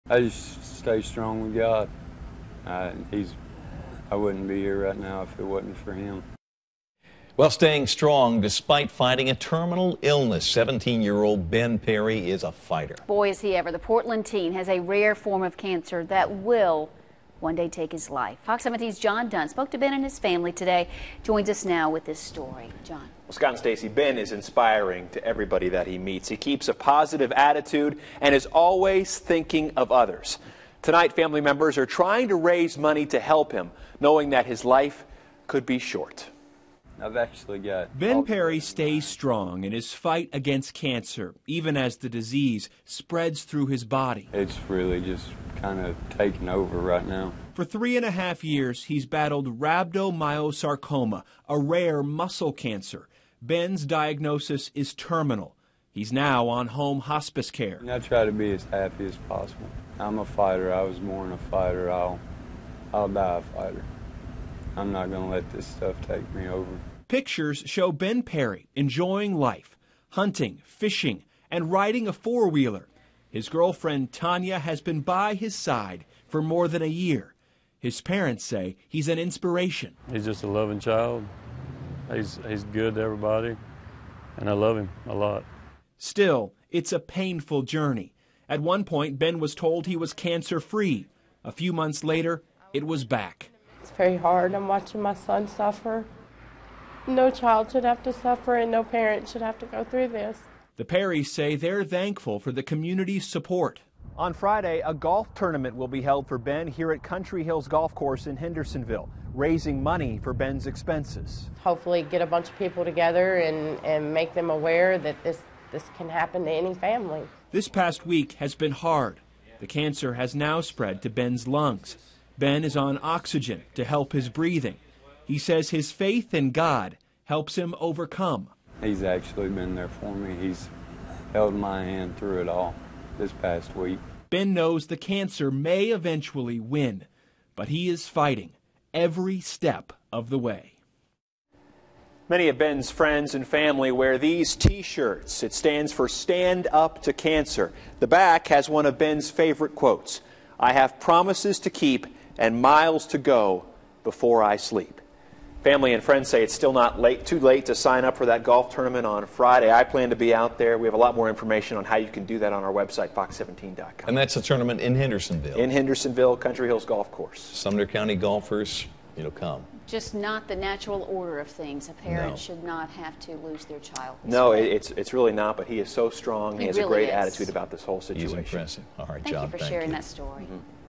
Portland Teen Fights Terminal Cancer -- John Dunn
Stream Fox 17 newscasts LIVE starting with Fox 17 This Morning at 5 am and News at 9 pm.

PORTLAND, Tenn. -- A Portland teenager is staying strong despite a terminal illness. 17 year old Ben Perry is fighter. Perry has a rare form of cancer that will one day take his life.

Ben is inspiring to everyone he meets. He keeps a positive attitude and is always thinking of others. Family members are trying to raise money to help him, knowing his life could be short. "It's really just kind of taking over right now," says Ben Perry.

For three and-a-half years he has battled rhabdomyosarcoma, which is a rare muscle cancer. Ben's diagnosis is terminal. He is now on home hospice care. "I try to be as happy as possible. I'm a fighter. I was born a fighter. I'll die a fighter. I'm not going to let this stuff take over," says Ben Perry.

Pictures show Ben Perry enjoying a life full of hunting, fishing, and riding a 4-wheeler. His girlfriend Tonya has been by his side for more than a year. His parents say he's an inspiration. "He's just a loving child. He's good to everybody, and I love him a lot," says Ben's father Albert Perry.

It is a painful journey. At one point Ben was told he was cancer free. A few months later, it was back. "It's very hard. I'm watching my son suffer. No child should have to suffer, and no parent should have to go through this," says Ben's mother Nickie Perry.

The Perry's say they are thankful for the community's support. On Friday a golf tournament will be held for Ben at Country Hills Golf Course in Hendersonville. It will raise money for Ben's expenses. "Hopefully get a bunch of people together, and make them aware that this can happen to any family," says Nickie Perry.

"Just about everybody in this town is trying to support me, and I really appreciate it," says Ben Perry.

This past week has been hard. The cancer has now spread to his lungs. Ben is on oxygen to help his breathing. He says his faith in God helps him overcome. "He's actually been there for me. He's held my hand through it all this past week," says Ben Perry.

Ben knows the cancer may eventually win, but he's fighting every step of the way.

Ben's parents say he'd like to have his own motorcycle, and would love the chance to meet a country singer like Justin Moore, Luke Bryan or Josh Turner.

Friday's golf tournament at Country Hills Golf Course in Hendersonville begins at 9:00 AM. The cost is $100 per player.

For news updates follow John Dunn on twitter @WZTVJohnDunn